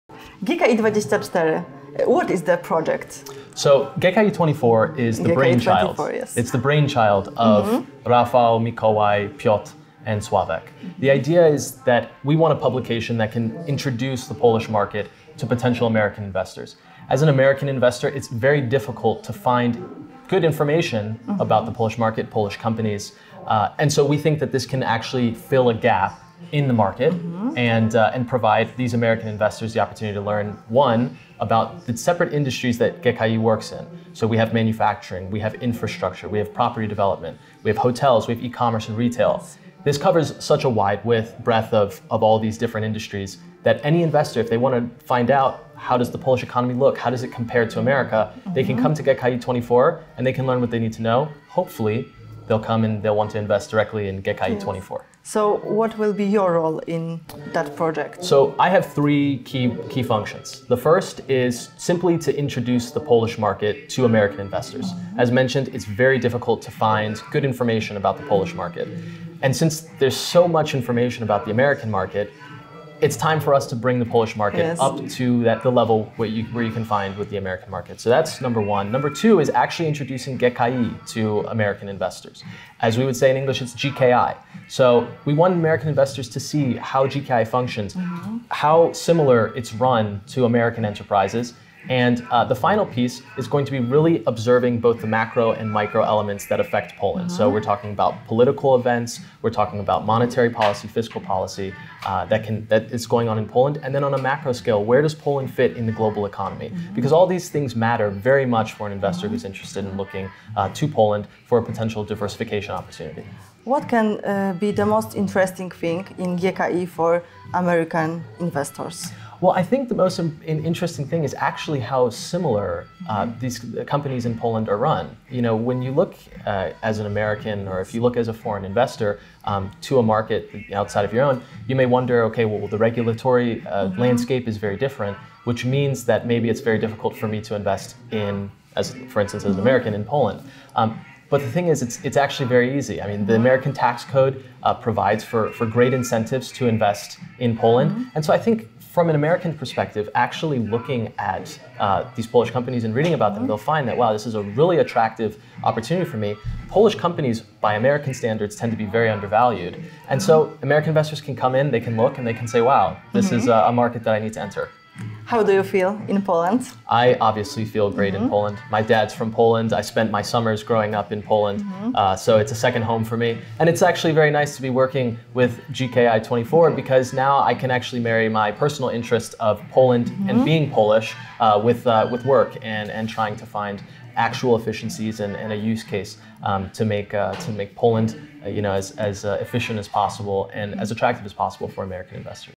Rozmowy na WZA. NICK MICHALAK: polskie spółki w amerykańskich standardach wydają się niedoszacowane
Podczas ostatniego Walnego Zgromadzenia Akcjonariuszy przeprowadziliśmy wiele inspirujących rozmów na temat inwestowania i rynku kapitałowego.

Jednym z naszych rozmówców był Nick Michalak, amerykański przedsiębiorca polskiego pochodzenia i korespondent portalu GKI24 w USA, który opowiedział o roli tego portalu w przedstawieniu amerykańskim inwestorom polskich spółek.

👉Tematy odcinka:
0:00 O projekcie GKI24
0:55 Rola w projekcie
1:53 Amerykański inwestor i polskie spółki
2:44 O Polsce